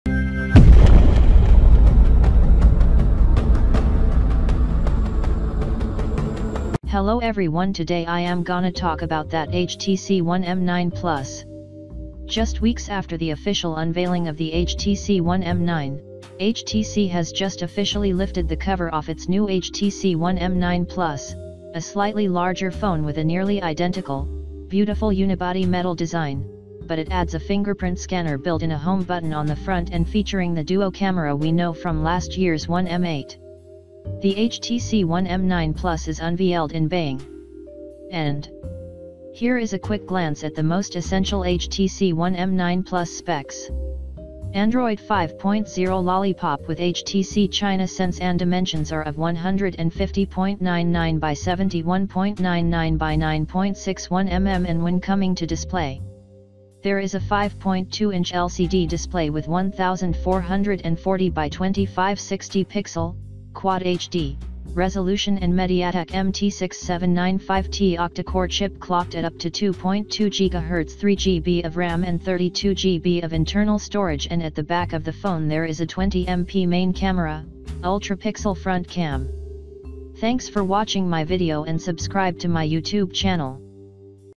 Htc One M9 Plus Hands On First Look 2015
Htc One M9 Plus Features a 5.2-inch LCD display with 1440 by 2560-pixel (Quad HD) resolution and MediaTek MT6795T octa-core chip clocked and specifications are at up to 2.2 Giga Hertz ,,3 G B of RAM and 32 G B of internal storage and at the back of the phone there is a  20   M P main camera, 'UltraPixel' front cam

Checkout My Channel  for Daily Tech Videos Such as News/Leaks/Concepts/Rumours/Reviews/Comparisions & Much Much More.